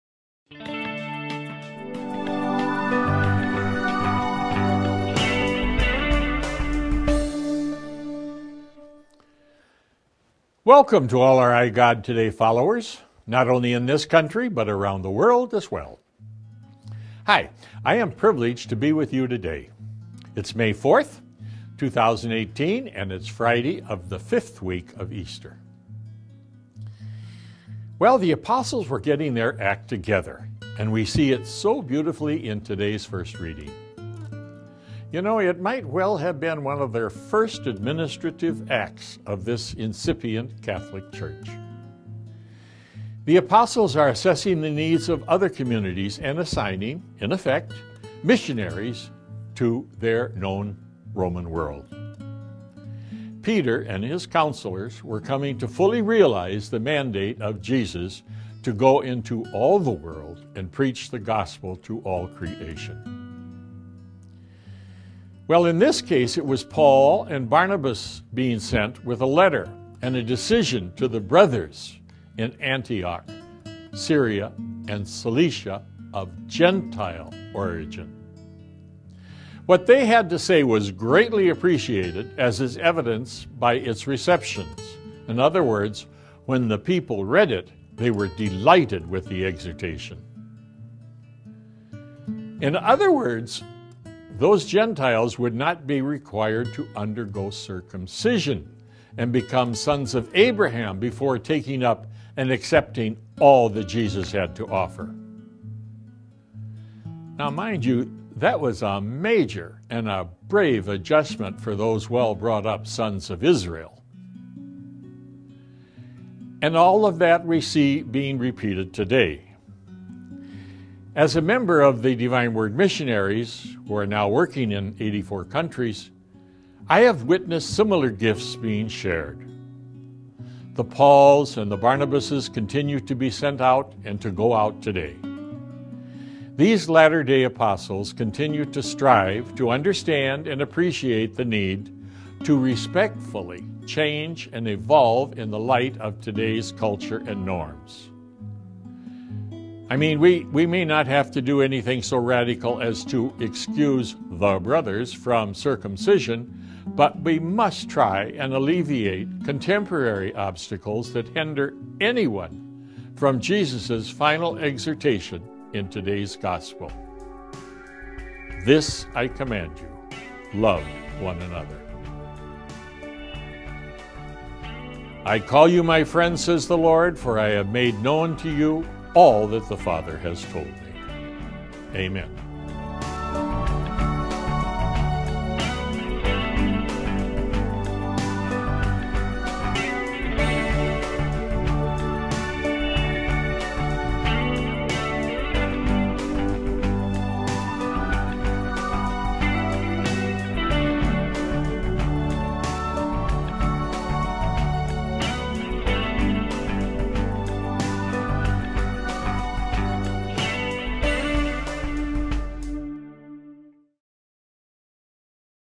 iGod Today with Br. Steve Pardy, SVD; Today's topic: “It is the decision of the Holy Spirit...
“I call you my friends, say the Lord.&nbsp; For I have made known to you all that the&nbsp; Father as told me!”&nbsp; (Jn 15:15B)&nbsp; Amen!